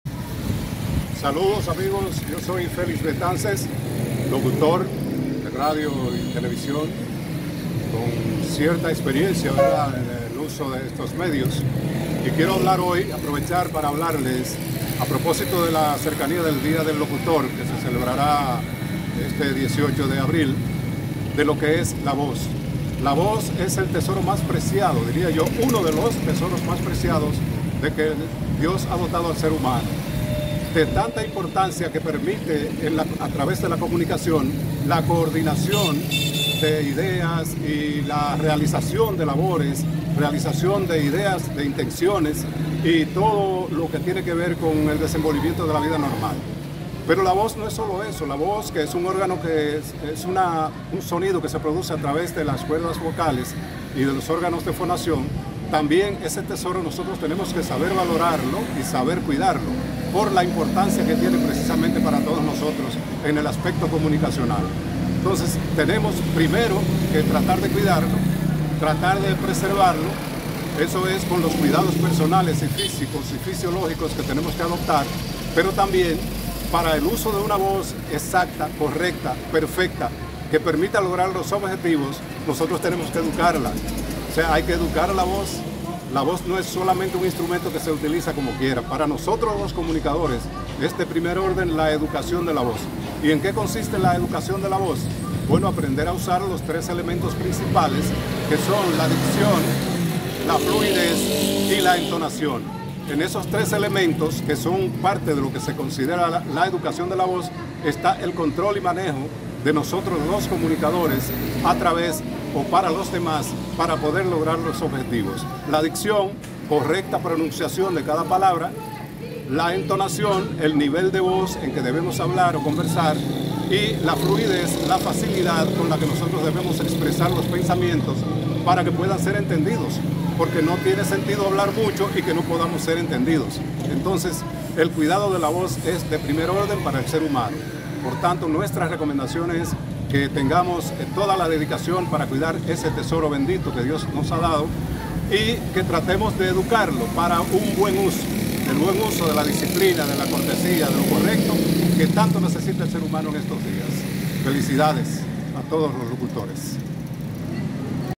Un maestro de la locución nos habla acerca de la voz y ofrece detalles que usted debe de aprender
El maestro de locución Felix Betances nos habla acerca de la voz y ofrece detalles del tema el cual cada persona debe de saber y aprender para comunicar de manera eficáz y tener un buen provecho de la voz, además aprovecha la ocasión para felicitar a los locutor al celebrarse este 18 de abril del año 2024 su día.

DARLE ME GUSTA
COMPARTIR
COMENTAR
ACTIVAR LA CAMPANA DE NOTIFICACIÓN